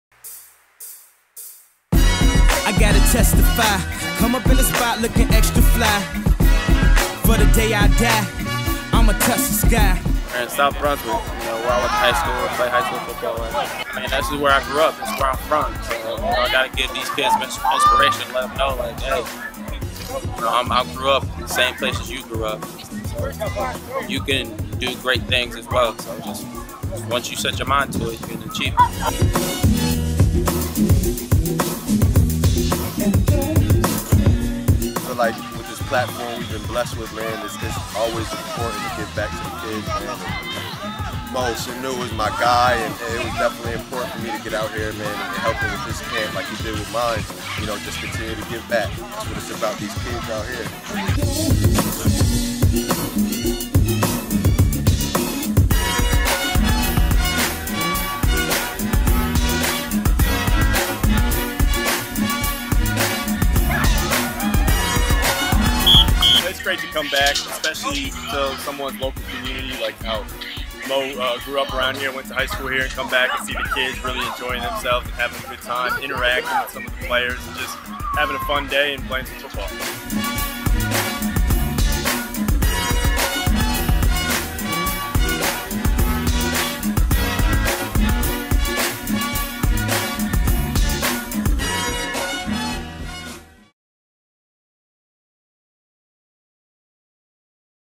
Mohamed Sanu Sr presents his 2nd annual "Sanu's Crew"
Mohamed Sanu Sr of the Atlanta Falcons presents his second annual "Sanu's Crew" event. Sanu holds his free football camp in his hometown of South Brunswick,NJ.